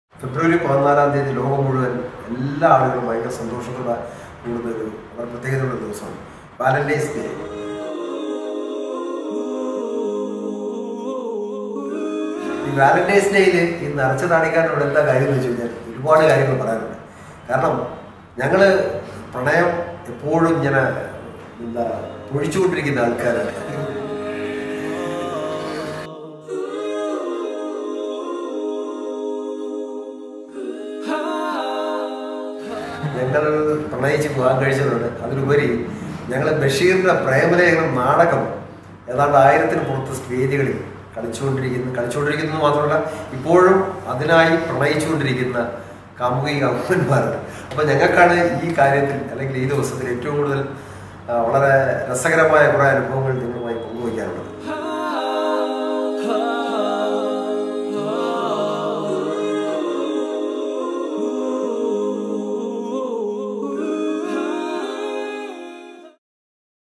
വാലന്റൈൻസ് ഡേയ്ക്ക് നരച്ച താടിയുള്ളവർക്ക് എന്താ കാര്യം എന്നാണോ?
‘വാലന്റൈൻസ് ഡേ എന്ന പേരിൽ പ്രണയികൾക്ക് ഒരു ദിനമുള്ള ഇക്കാലത്ത് നരച്ച താടിയുള്ളവർക്ക് എന്താ കാര്യം എന്നല്ലേ... പറയാം’